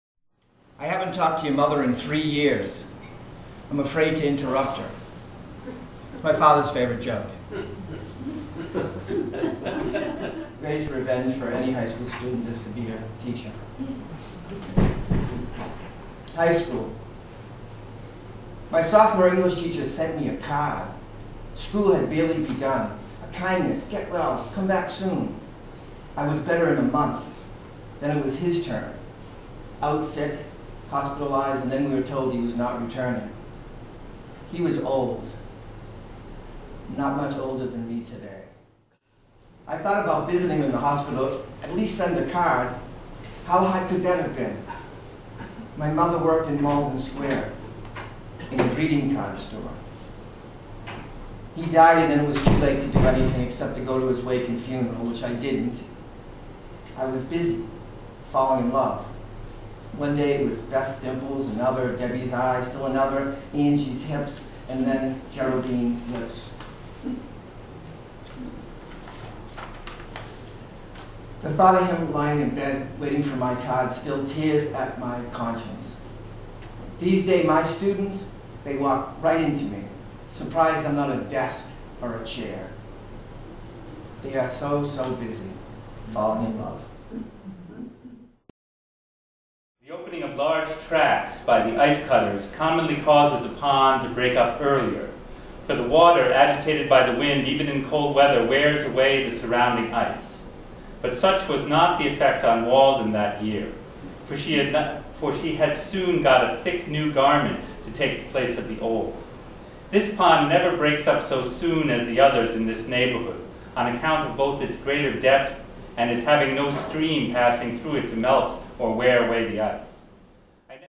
Poetry for The Spring Equinox
Poet David Surette reads at Poetry for the Spring Equinox, part of the Walden Poetry Series, at Concord Free Public Library, Concord, Mass. March 21, 2009.